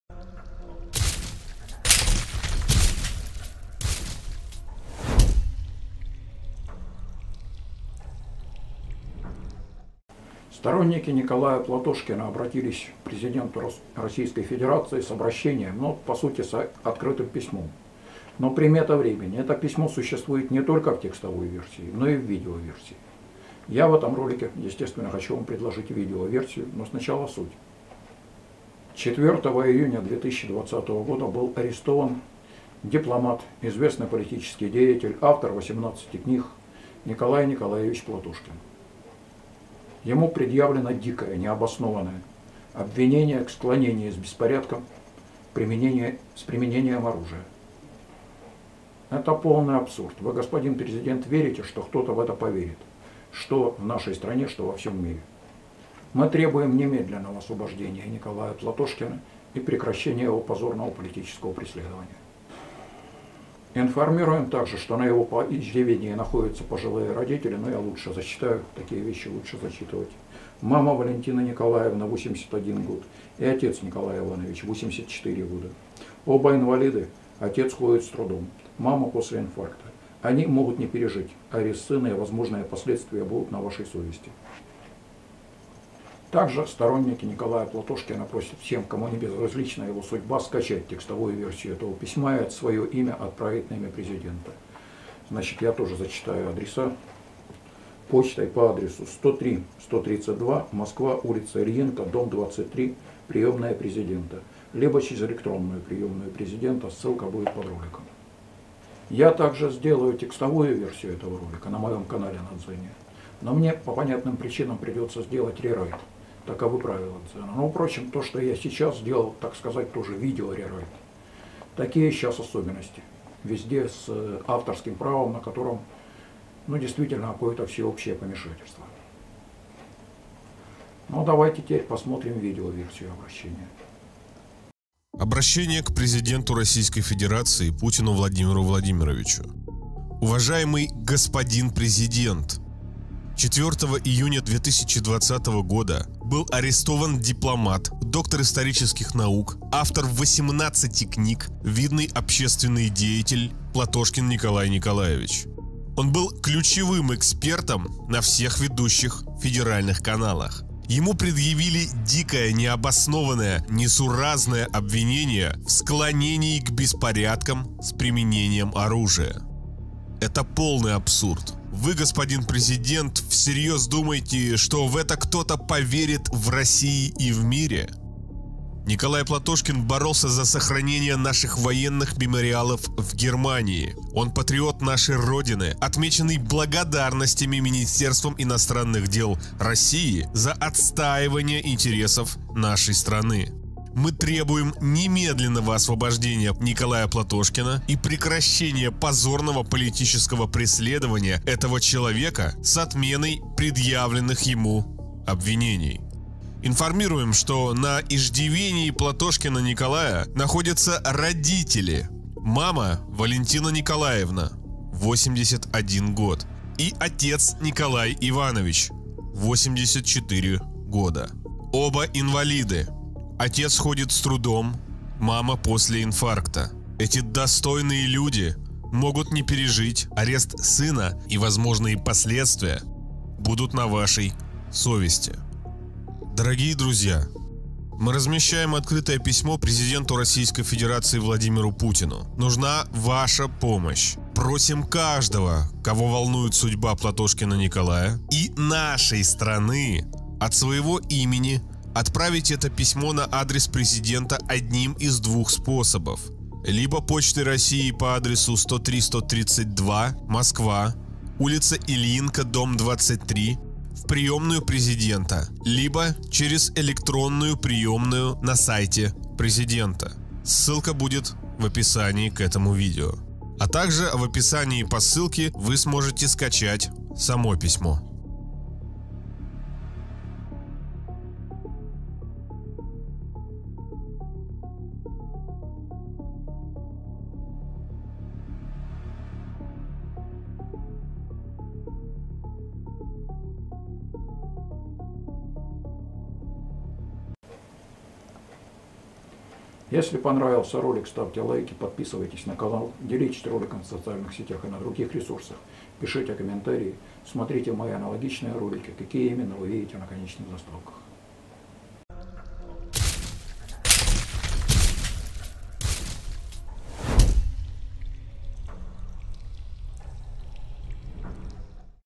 Обращение к президенту РФ по поводу ареста Н. Н. Платошкина
Направить обращение на имя Президента РФ
Отправить Почтой России:103132, Москва, ул. Ильинка, д.23

Ссылки:

Мой канал на Дзен, он для мене не менее важен, чем канал на Ютубе